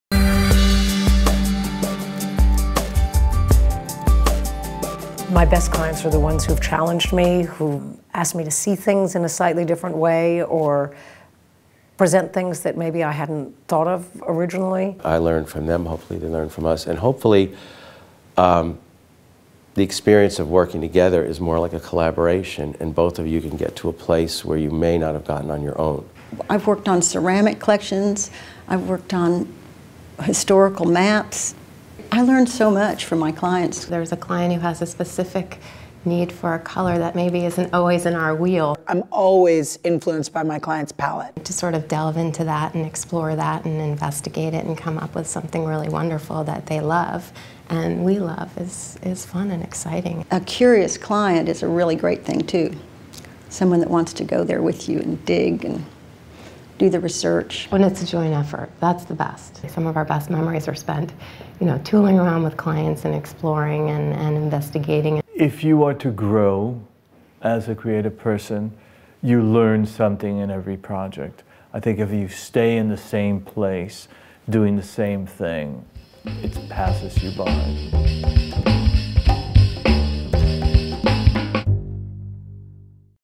2012 AD100: How Do Your Clients Influence Your Work?
Hear from select members of the 2012 AD100 and find out why Martyn Lawrence Bullard always questions a client before he agrees to a project; why Mario Buatta believes a decorator must also be an actor, a psychiatrist, and a lawyer; why Bunny Williams thinks a great home is like a perfectly made suit; and much more

2012 AD100: How Do Your Clients Influence Your Work?